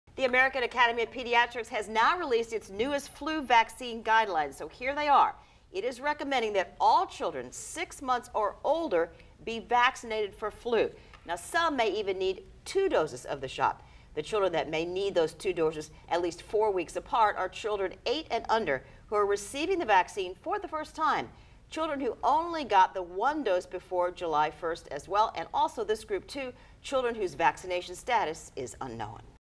New flu vaccine guidelines
The American Academy of Pediatrics released it's newest guidelines.